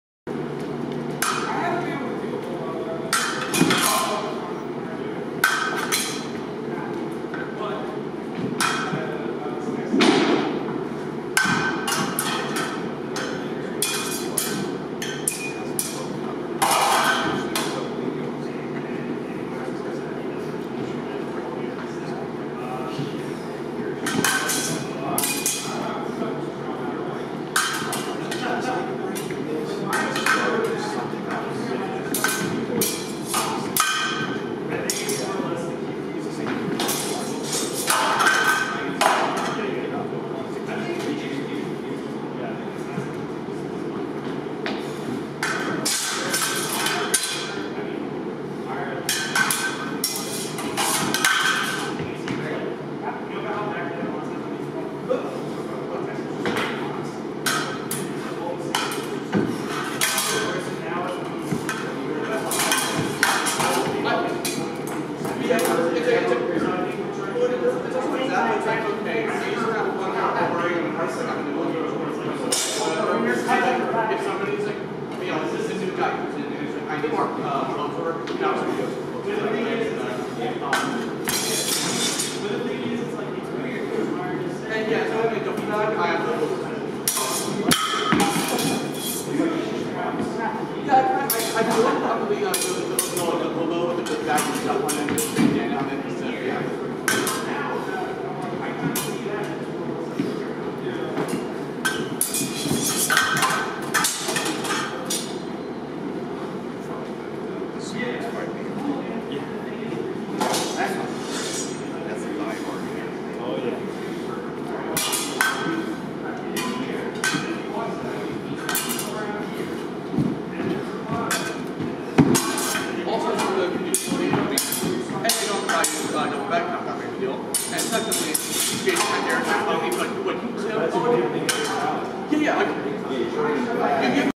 Jeff and Ryan sparring
Sparring with my new Talhoffer buckler and not so new sidesword